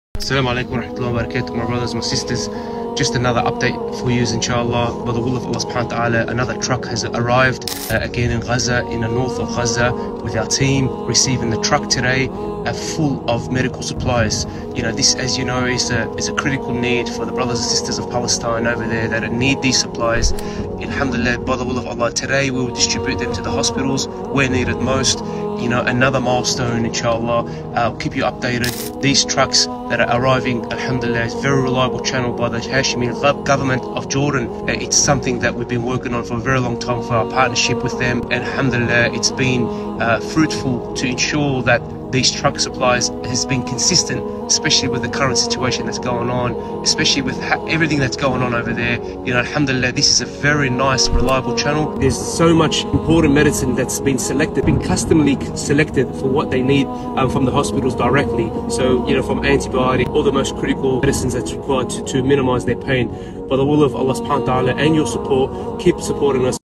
Live Update from Palestine
Addressing the critical needs of our brothers and sisters in Palestine, we have sent a medical truck via Al Hashimi Charity Organisation in Jordan to bring relief to those in need. We will continue sending relief to Palestine in every way possible. Keep donating.